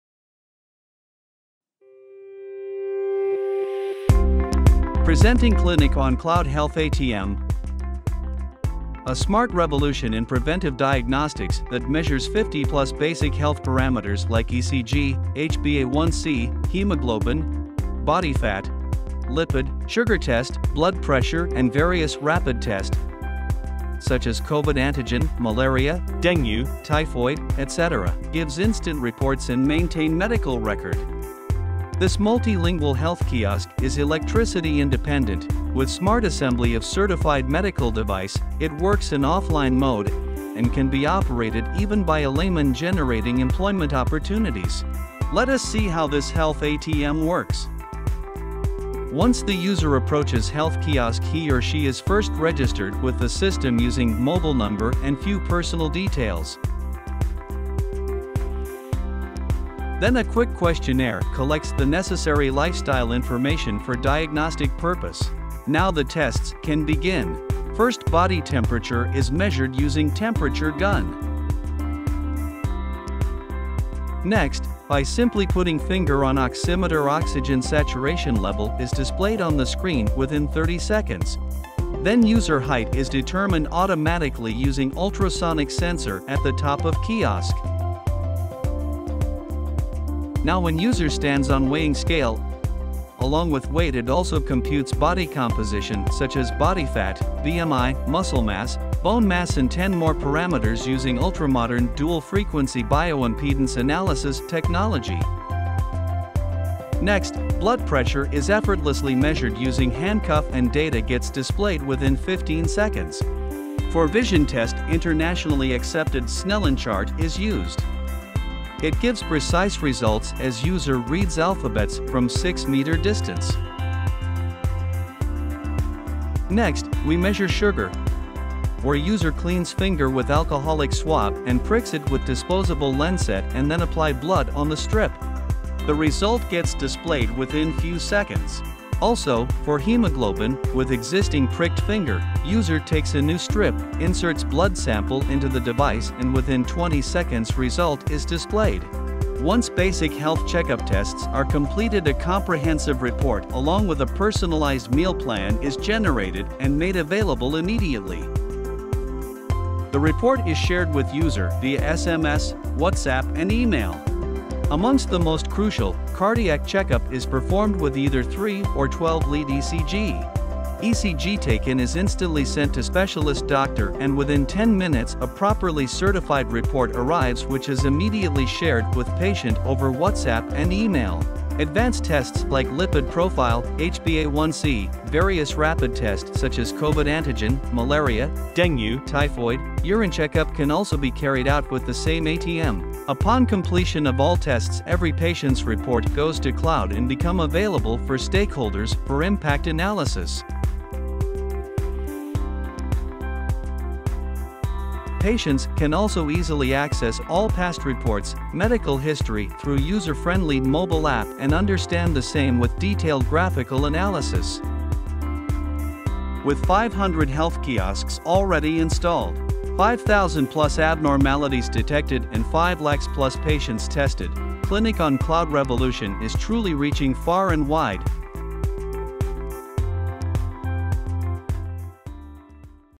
Health ATM - Health Kiosk - Product Video
Clinics On Cloud Health kiosk is an aggregation of CE/FDA/medical grade
 devices combined with HIPPA complaint backend software which solves
 problem of basic health awareness checkup in rural/urban areas.